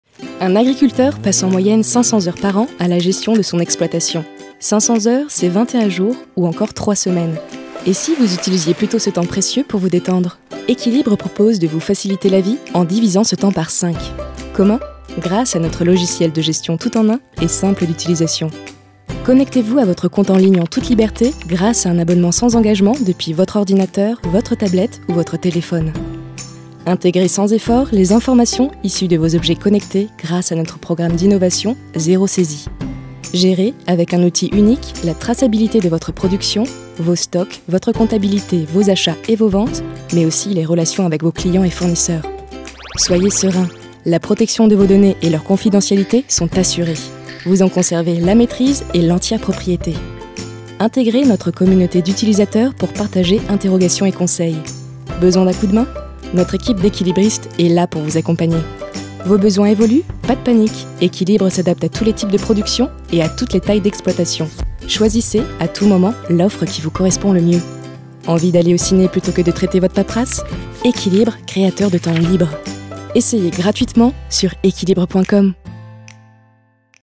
Ekylibre - le logiciel "tout-en-un" au service de l'agriculture performante
Ekylibre - logiciel de gestion d'exploitation pour l'agriculture de demain qui a pour objectif de libérer les agriculteurs de leurs charges administratives (500h en moyenne par an). Divisons ce temps par 5 ! Comment ? Grâce à un outil de gestion complet, interopérable, open source et innovant.
Ekylibre, créateur de temps LIBRE !